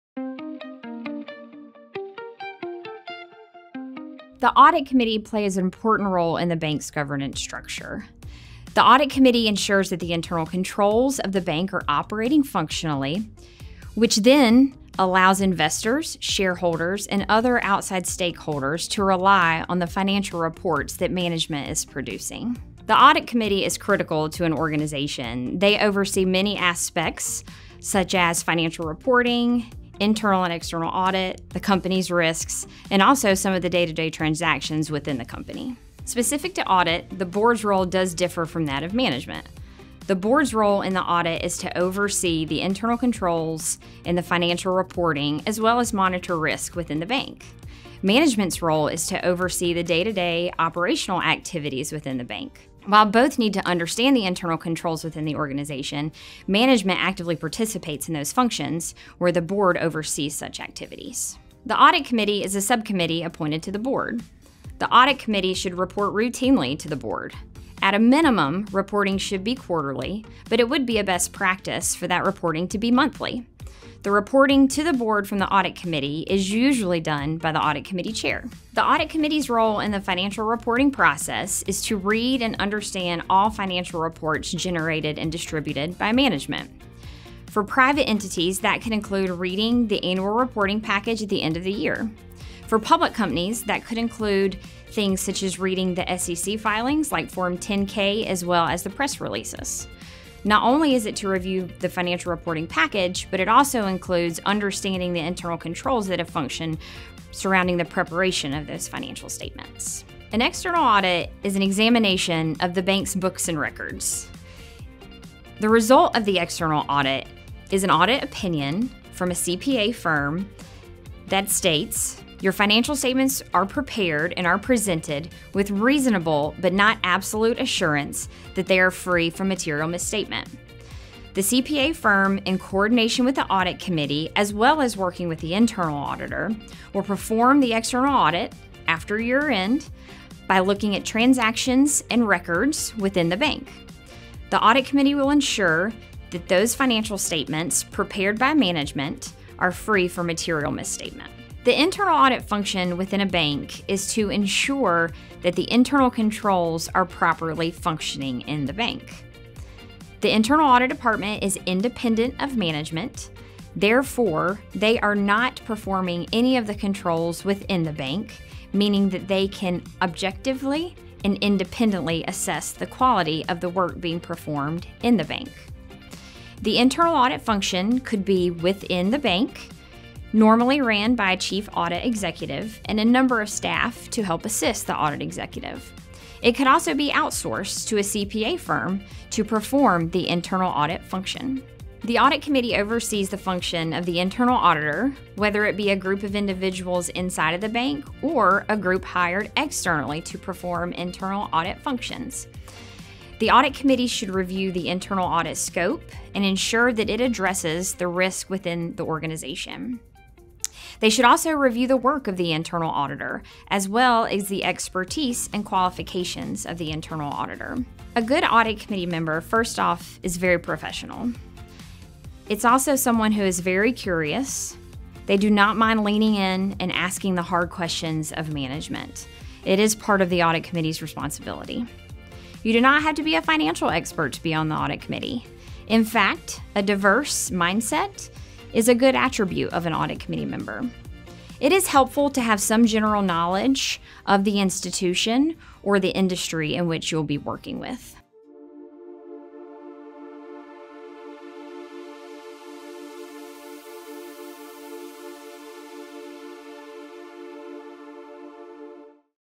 Audit Fundamentals the Board Should Know
In this video, Forvis Mazars’ Ashley Ensley breaks down audit committee responsibilities and the makings of a good audit committee member.